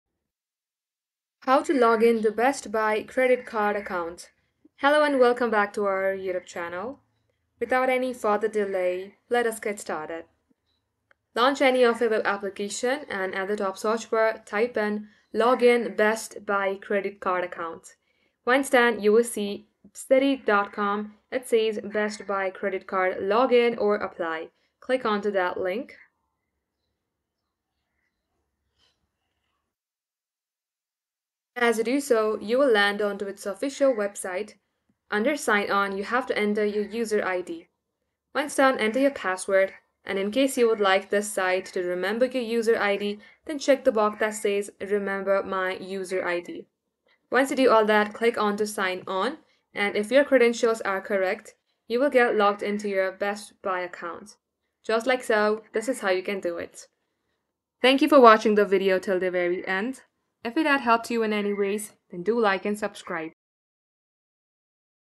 How to Login Best Buy Credit Card Account - BestBuy.com Login Credit Card [Easy Guide]
How to Login to Your Best Buy Credit Card Account | BestBuy.com Login Credit Card [Easy Guide]

Need to access your Best Buy Credit Card account? Follow this straightforward guide to log in to your account quickly and easily.

Timestamps:
0:00 - Introduction
0:10 - How to Login to Your Best Buy Credit Card Account | BestBuy.com Login Credit Card [Easy Guide]

Key points covered in the video:
- Explanation of why you might need to log in to your Best Buy Credit Card account.
- Step-by-step instructions for logging in to your account on BestBuy.com.
- Tips for resetting your password if you encounter login issues.
- How to check your account balance, view statements, and manage your credit card online.